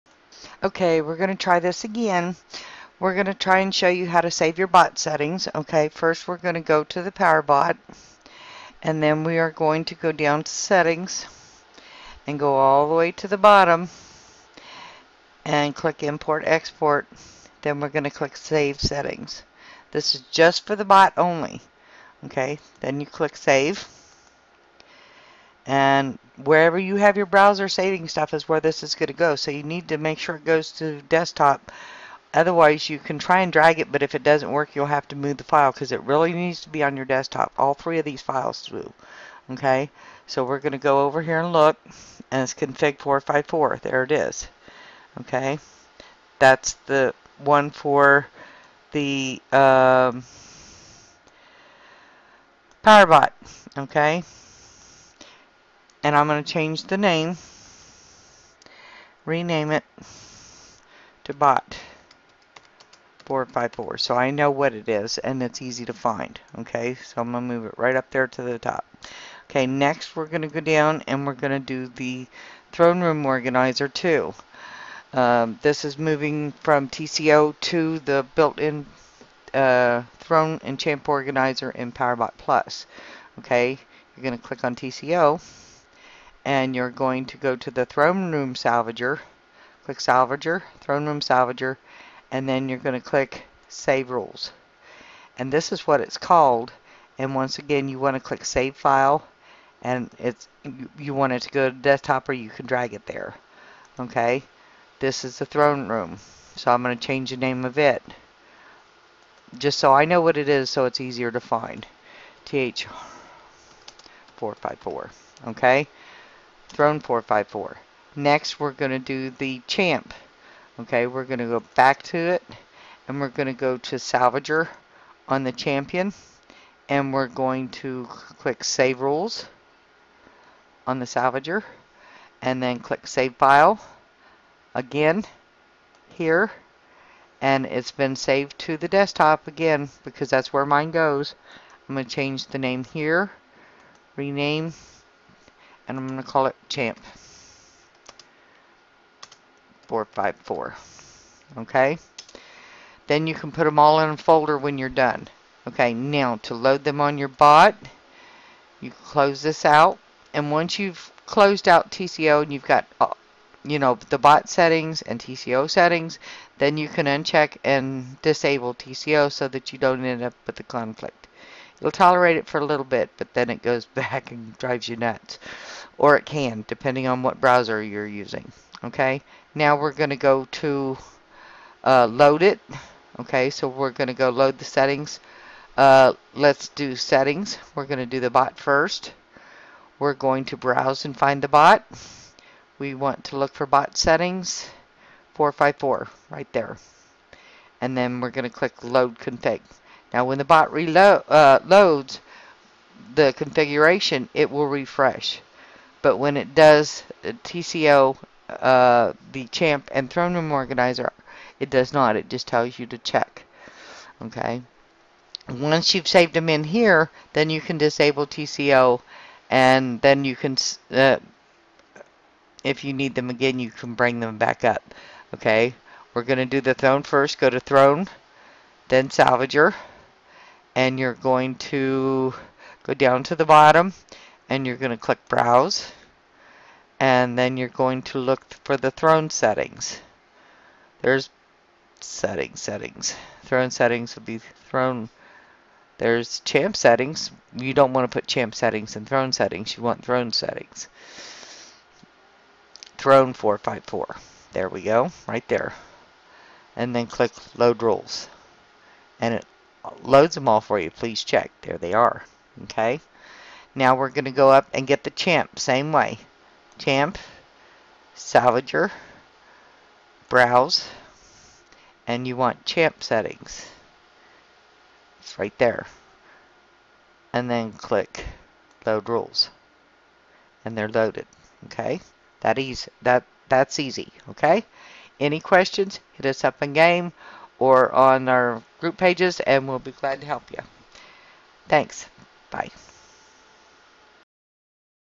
bot throne and champ save
how to save ur setting  on power bot plus and  throne and champ